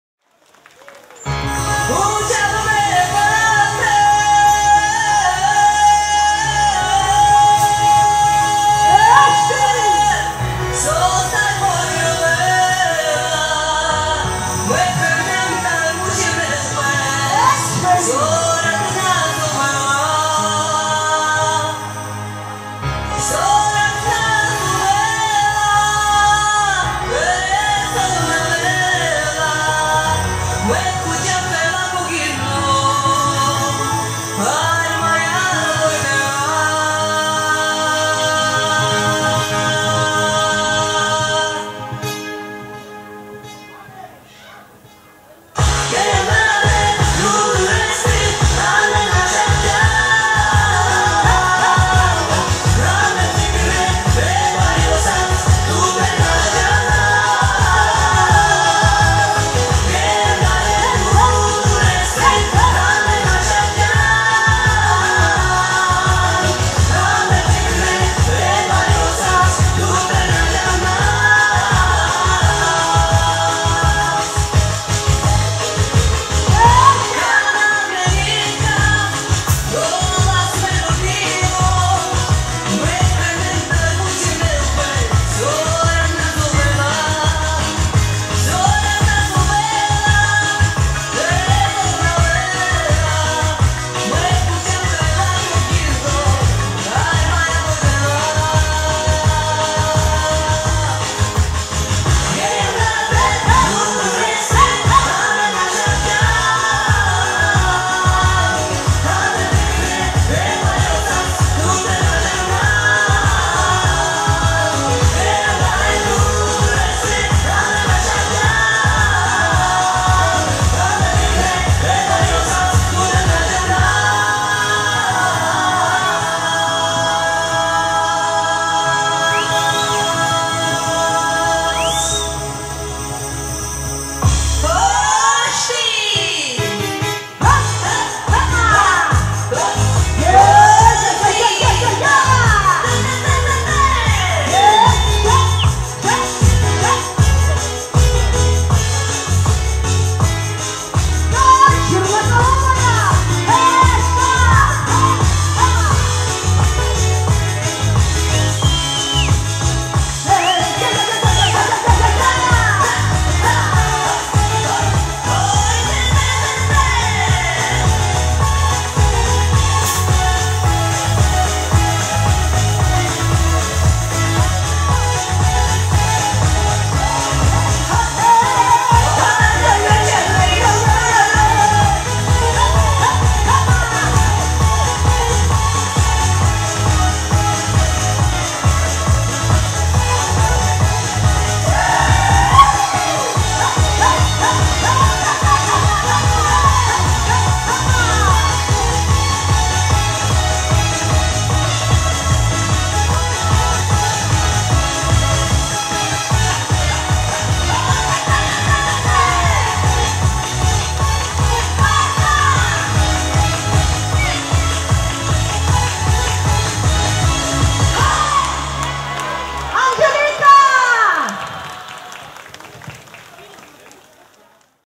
Liza - część 17. V Festiwal Kultury Romskiej w Lubaniu.
V Festiwal Kultury Romskiej w Lubaniu - relacjonuje Bielawa Dolna. Wspaniała zabawa. Więcej informacji i zdjęć z V Festiwalu Kultury Romskiej w Lubaniu na naszej stronie: